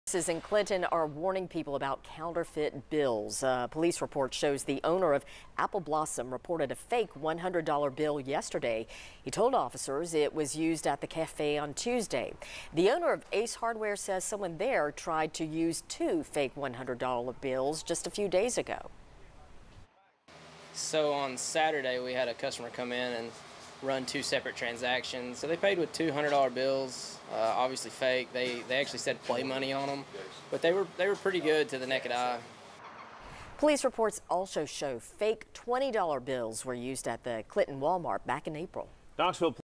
Thieves using counterfeit bills at some Clinton stores
Apple Blossom Café and a Walmart in Clinton said they found counterfeit bills that look almost exactly the same as legitimate bills.